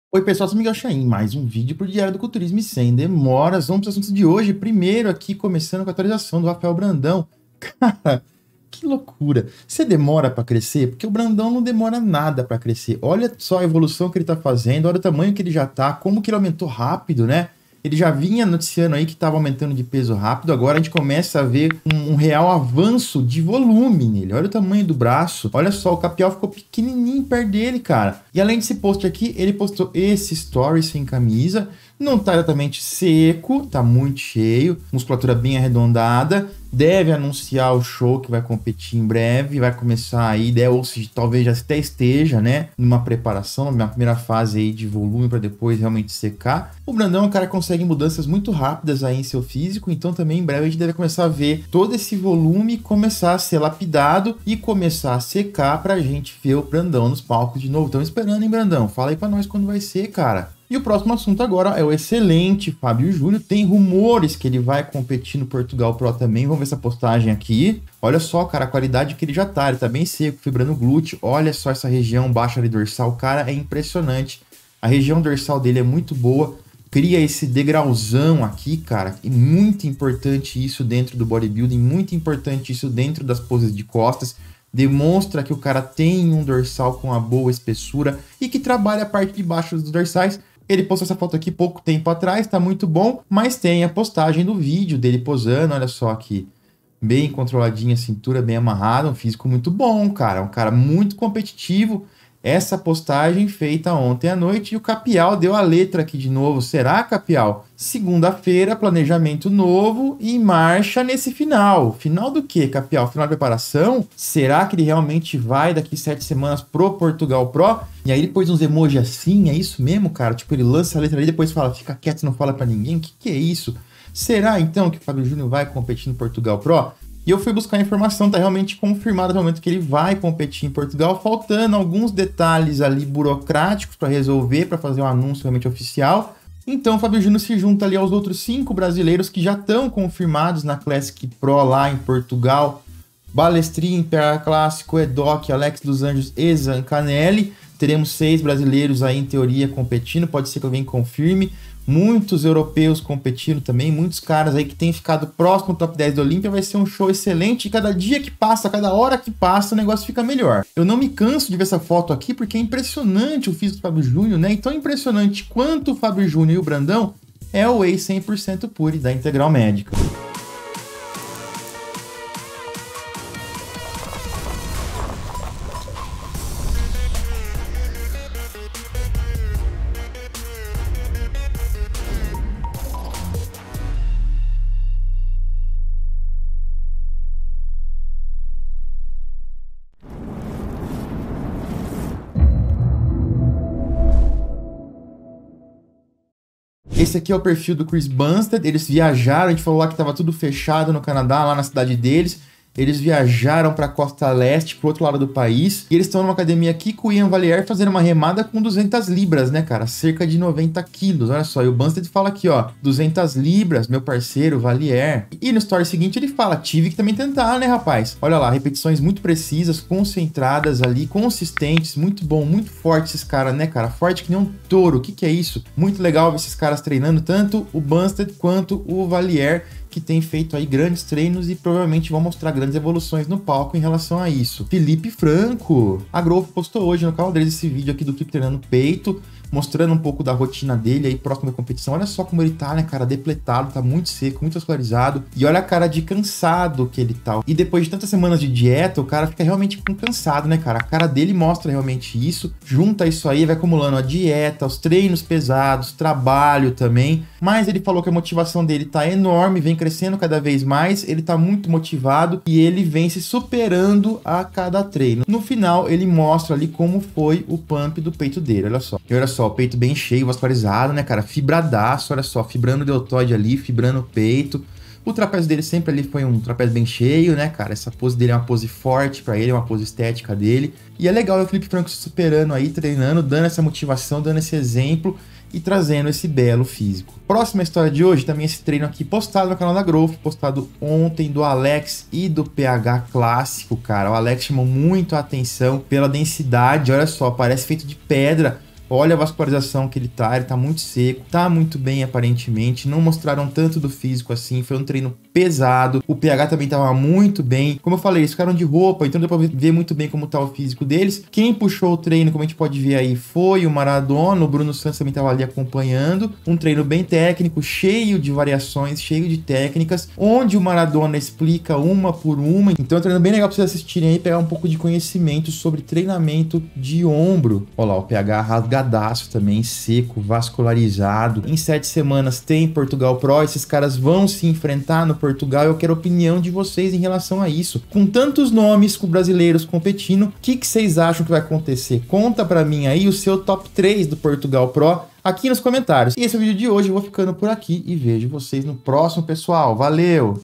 OFF de Brandão chegou ao fim? +Alex dos Anjos e PH treinam juntos + Franco: Melhor shape da vida?
Whey 100% Pure Integralmedica

Curso Montagem e Periodização de Treinamento  (mais de 600 inscritos)
em até 12X

Content:
00:00 - Rafael Brandão
01:00 - Fabiojunio no Portugal Pro
02:49 - Integralmedica
03:16 - CBUM
03:54 - Felipe Franco
04:47 - Alex e PH Classico